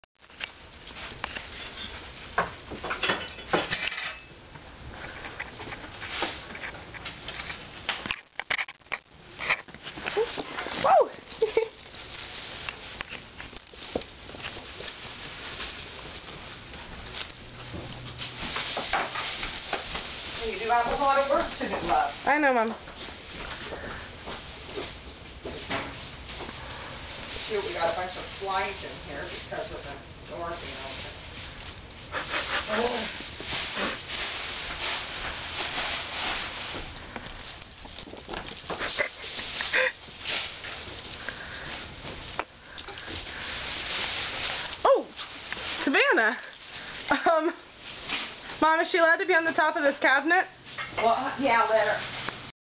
Savannah Plays With the Curtains
My new kitten Savannah playing with our beaded curtains